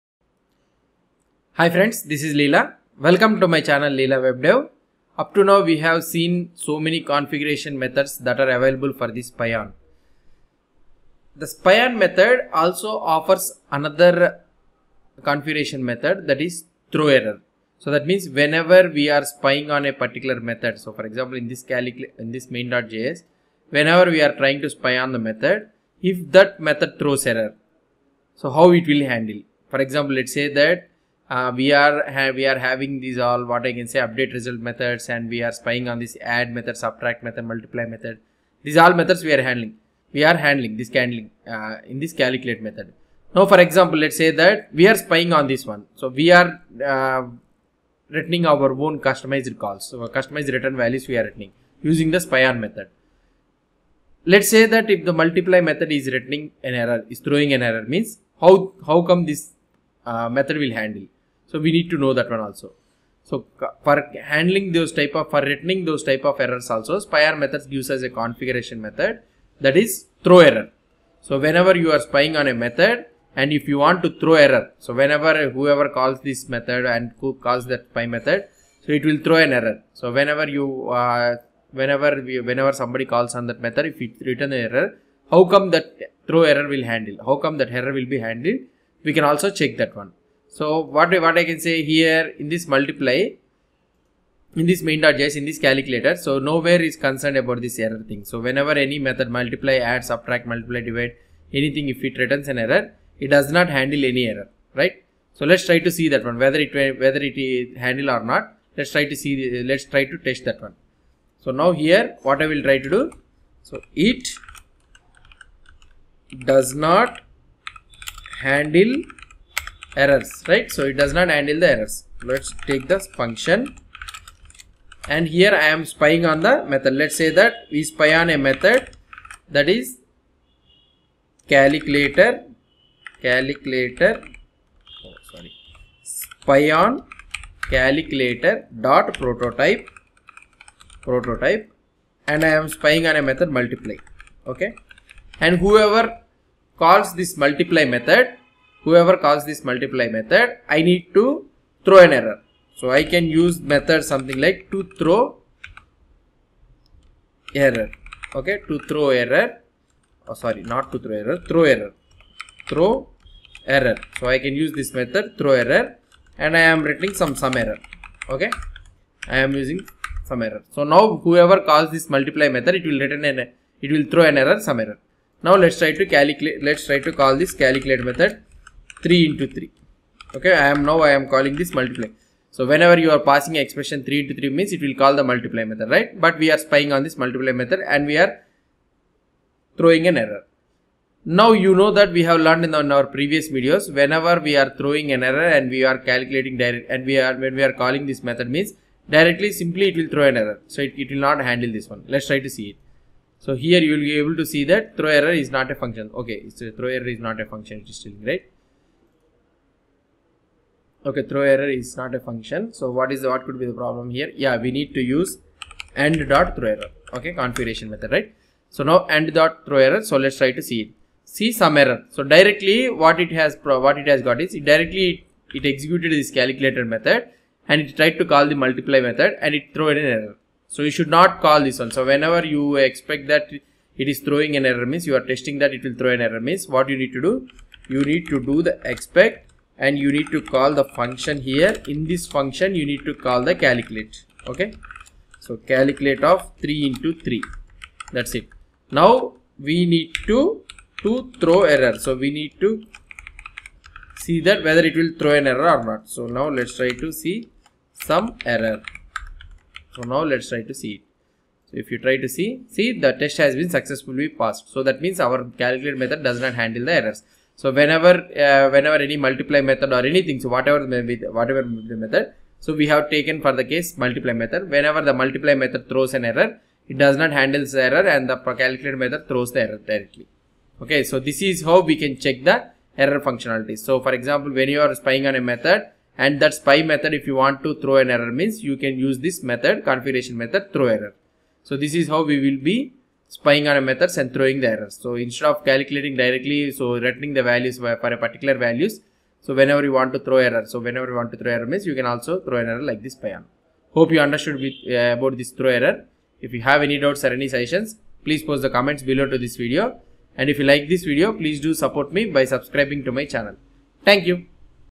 50. throwError config method to send error when a spy method is called - Jasmine Testing
In this video we will see how to send error for a spy method using throwError - Jasmine Testing